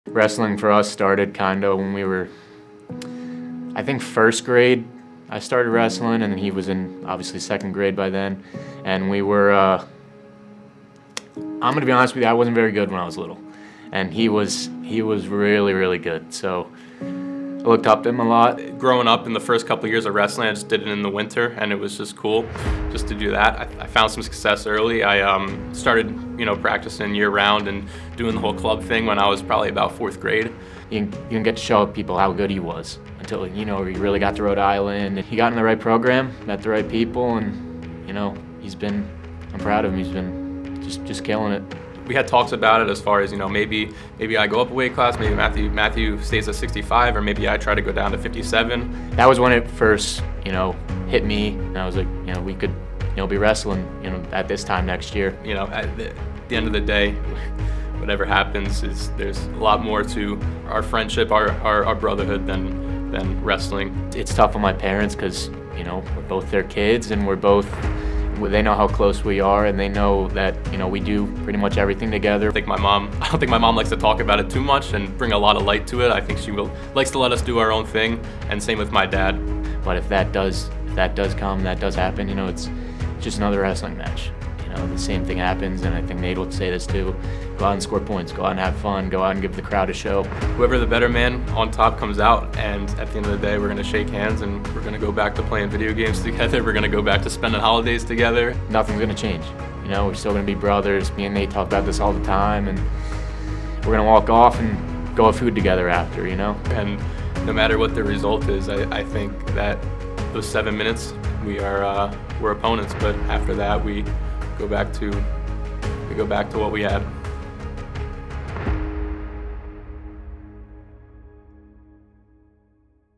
NCAA Wrestling Feature: Nathan Lackman & Matt Lackman
For Nathan and Matt Lackman, wrestling has strengthened the bonds of brotherhood, quite literally. On Saturday, March 11, 2023, the duo made history, squaring off against one another in the finals for the 165# title at the 2023 NCAA Division III Wrestling Championships.